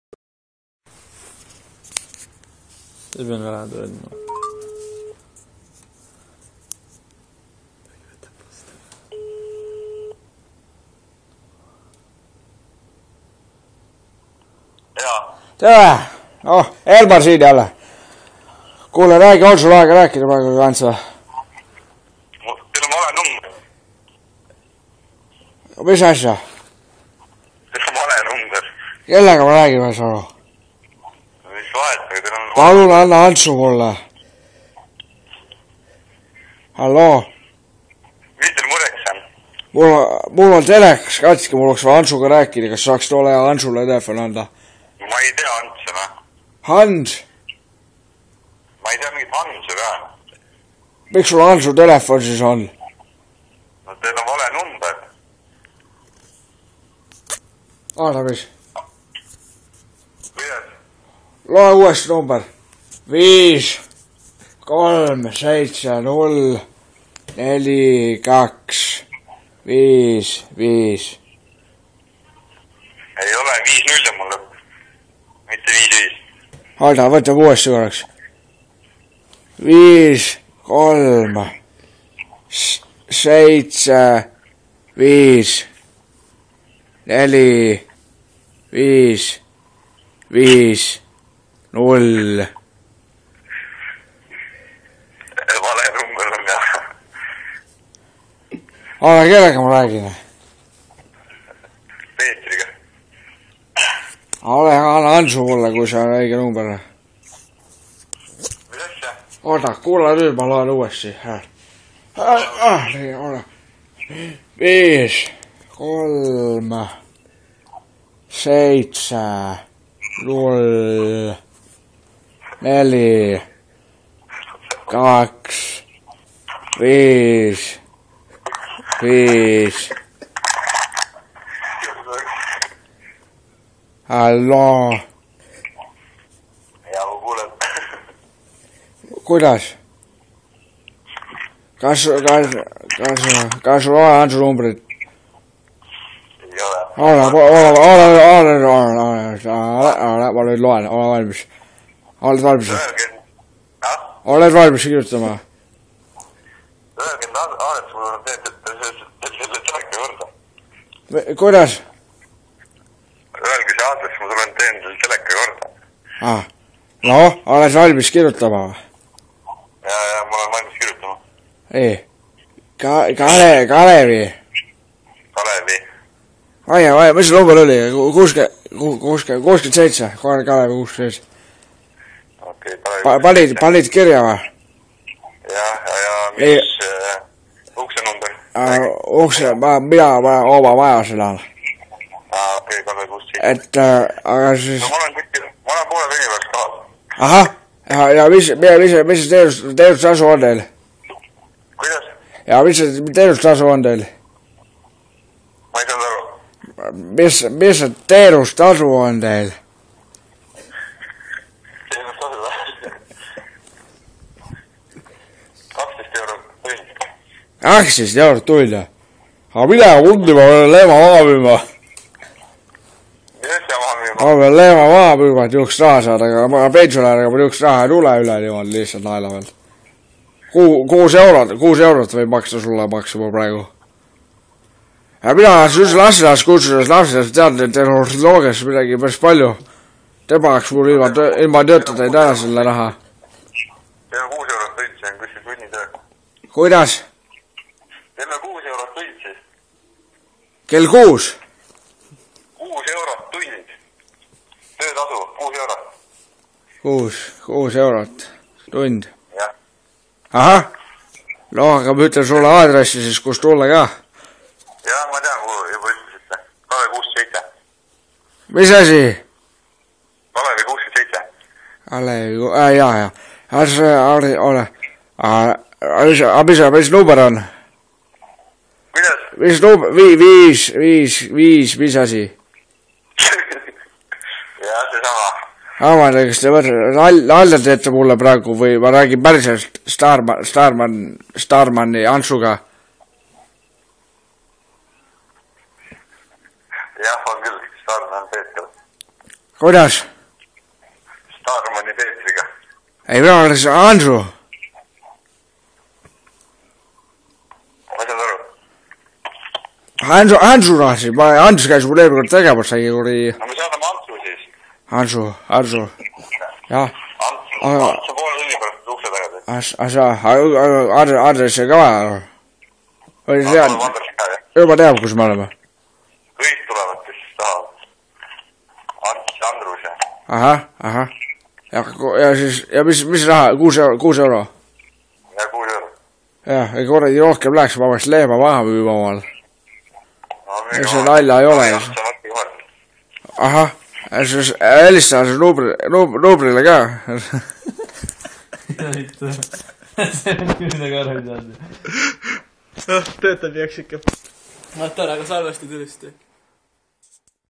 Vanaisa helistab Starmani Hansule
Üks õhtu kui vanaisa lapselapsed Wii-ga mängisid lendas üks pult telekasse ja telekas läks katki, Vanaisa helistab seejärel Hansule, kes tal telekat eelmine kord parandas. Kahjuks on Hansul veel eelmisest õhtust kõva pohmakas ja ta arvab, et ta ei ole Hans. Vaata, mis saab edasi ja mille peab vanaisa peaaegu maha müüma.